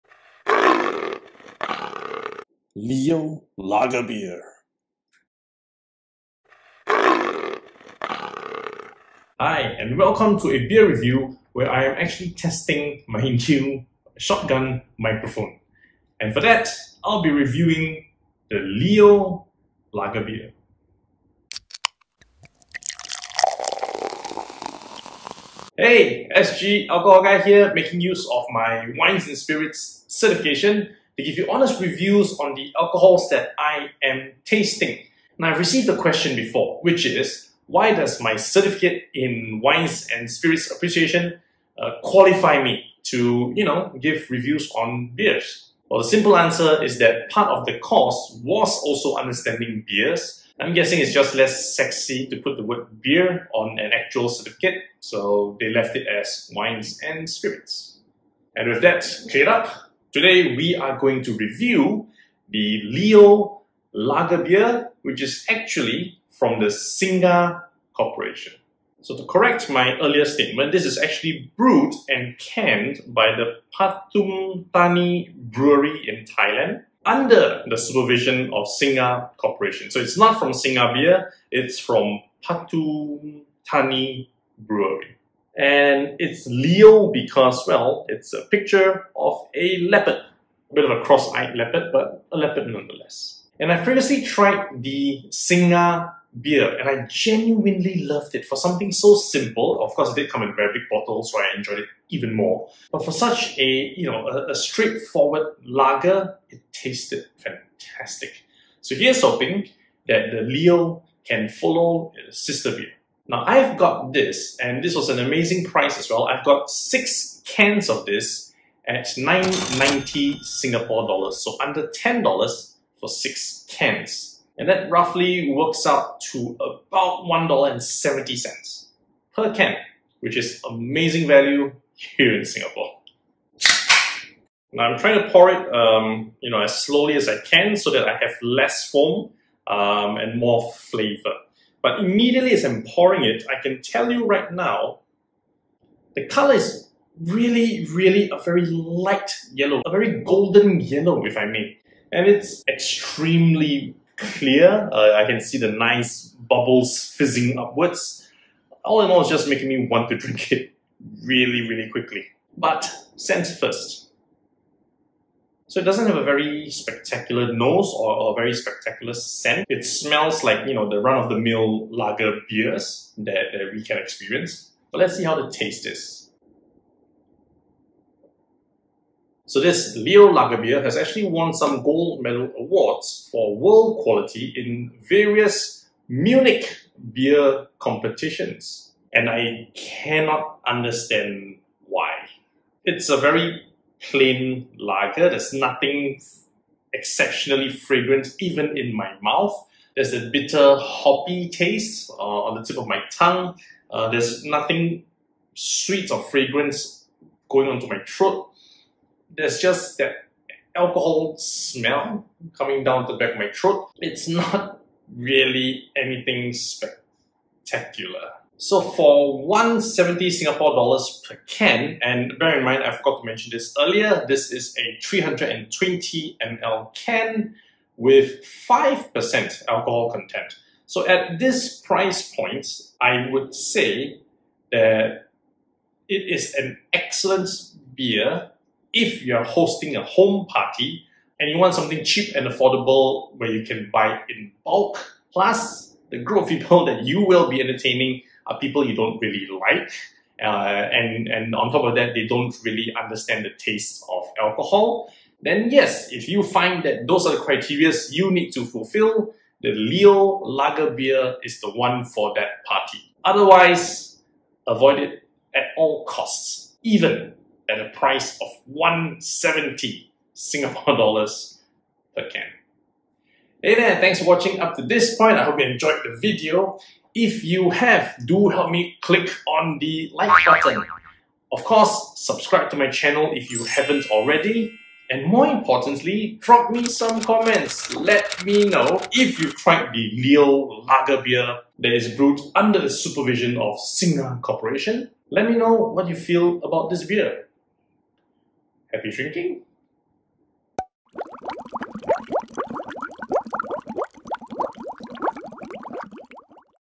Leo Lager Beer - Honest Review
Leo Lager Beer review from a certified alcohol expert! Check out my taste test to see if this drink suits your palate.